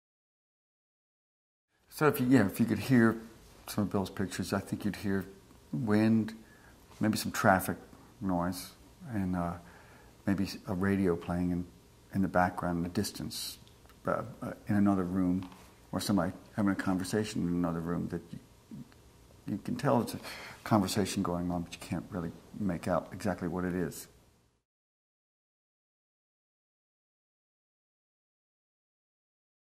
David Byrne on William Eggleston
Musician David Byrne talks about the music and audio qualities found in the visual art work of artist William Eggleston.

The former Talking Head's singer hears wind and traffic in the photography of Eggleston.

David Byrne Interview part 3